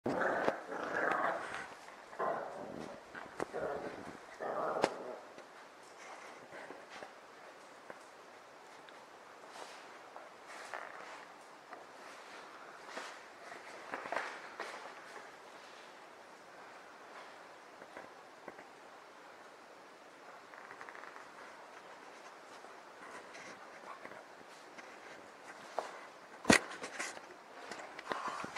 Merlsheimer Welpen spielen mit einem Baumwollseil
Merlsheimer Welpen (kleinste Variante) spielen mit einem Baumwollseil-Ball. Ein Welpe springt in die Kamera.